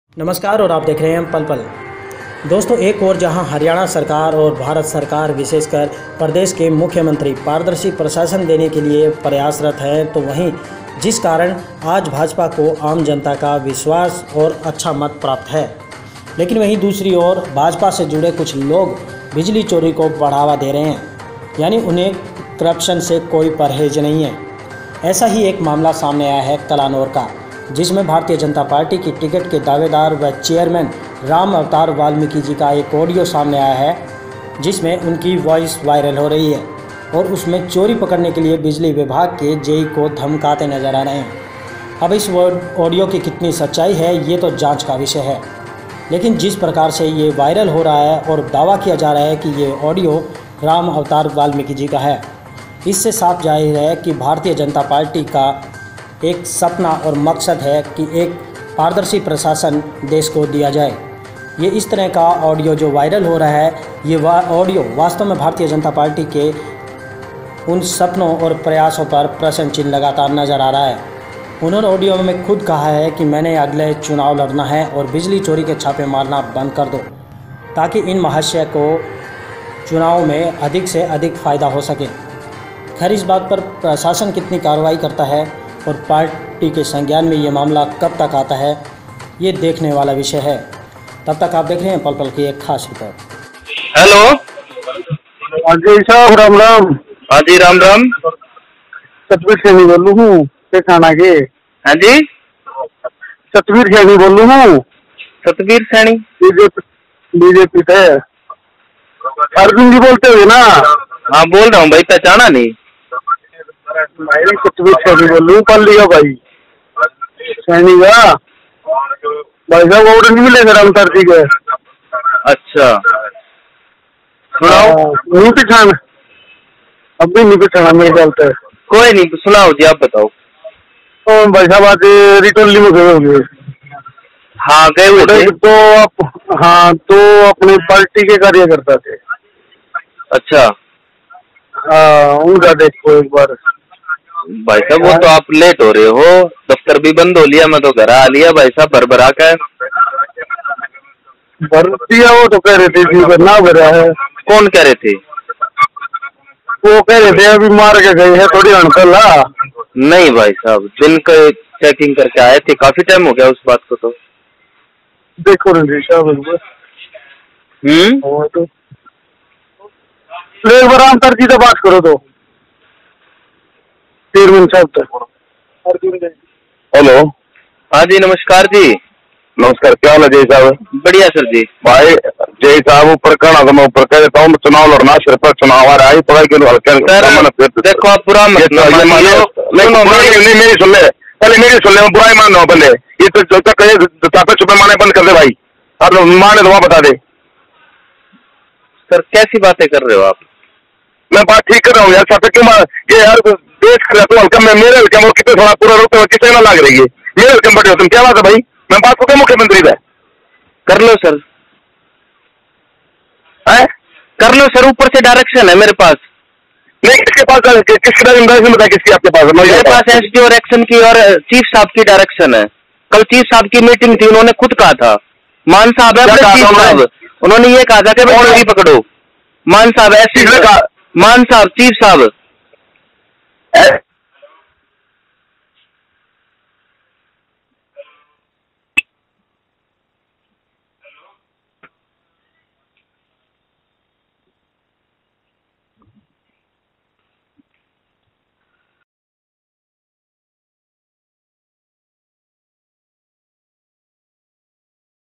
बिजली चोरी पकड़ने वाले J.E को धमकाते हुए एक चेयरमैन सुने ?
एक और हरियाणा सरकार विशेषकर प्रदेश के मुख्यमंत्री पारदर्शी प्रशासन देने के लिए प्रयासरत हैं जिस कारण आज भाजपा को आम जनता का विश्वास भी प्राप्त हो रहा है वहीं भाजपा से जुड़े कुछ लोग बिजली चोरी को बढ़ावा दे रहे हैं ऐसा ही एक मामला कलानौर से भारतीय जनता पार्टी के टिकट के दावेदार वह चेयरमैन रामअवतार बाल्मीकि का सामने आया है इन दिनों उनका एक वॉइस वायरल हुआ है जिसमें वे चोरी पकड़ने गए बिजली विभाग के जेई को धमका रहे हैं आप सुने क्या कह रहे हैं रामअवतार बाल्मीकि